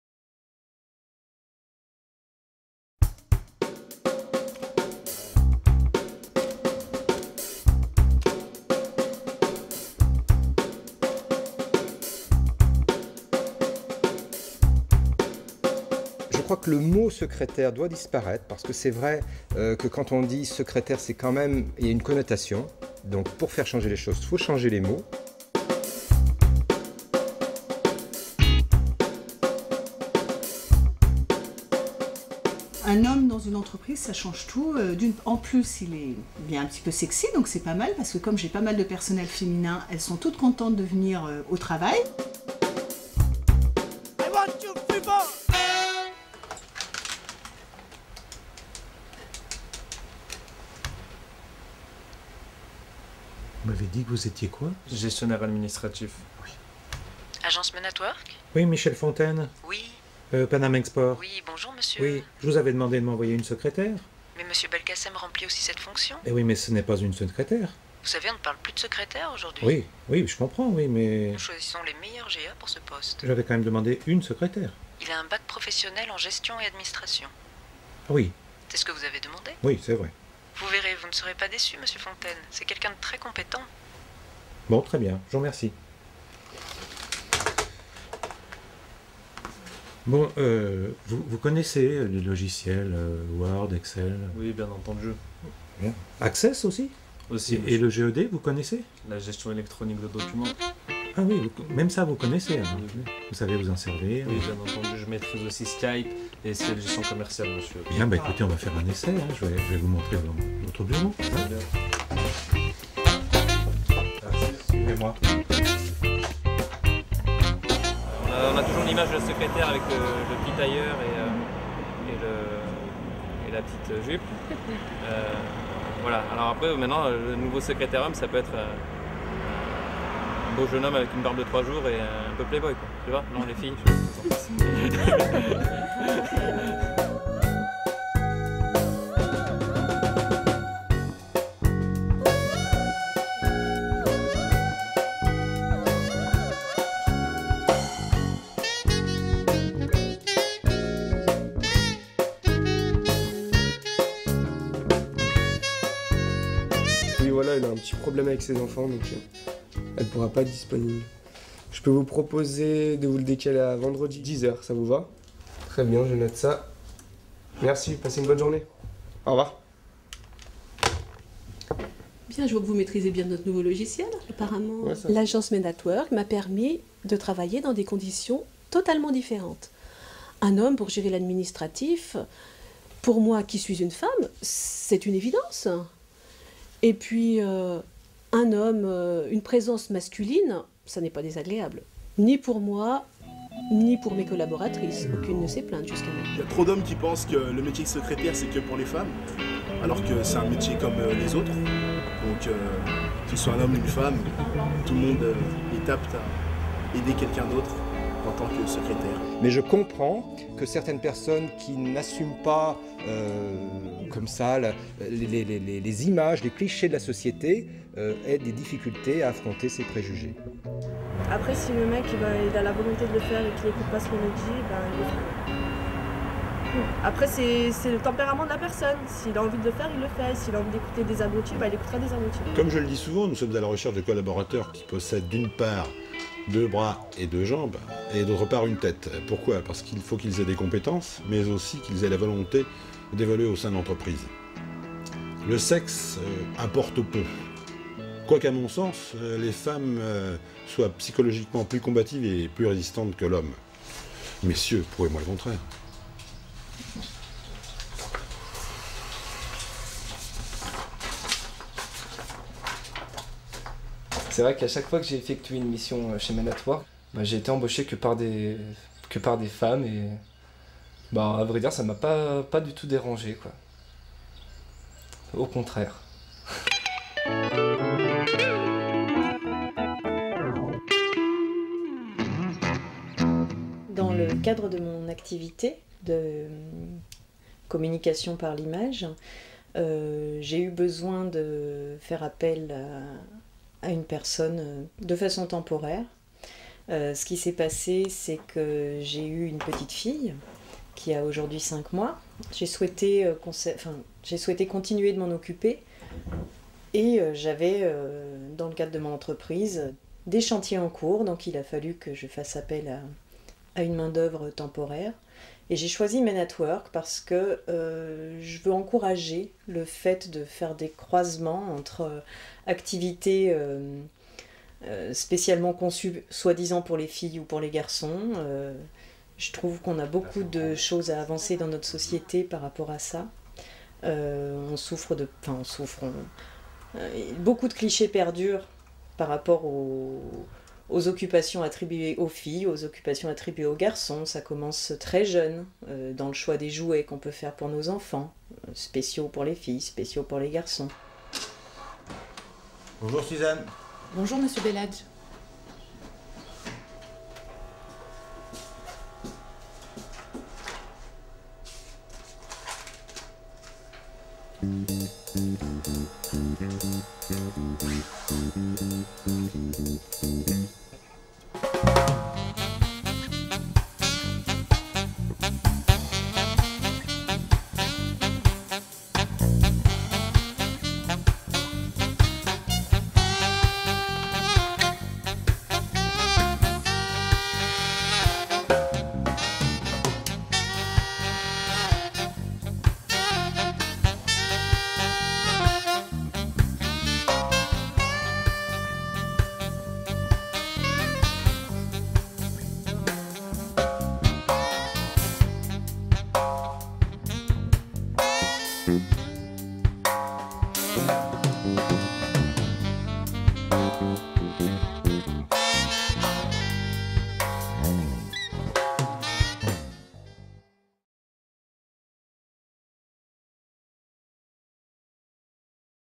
Men at work - vidéo humoristique
Des élèves de gestion-Administration, un scénario, un métier et beaucoup d'humour... Men at work, une agence d'intérim qui gomme les clichés

Un homme secrétaire, un beau jeune homme... Si pour vous aussi, il faut que ça change ! Des collaborateurs efficaces tout simplement.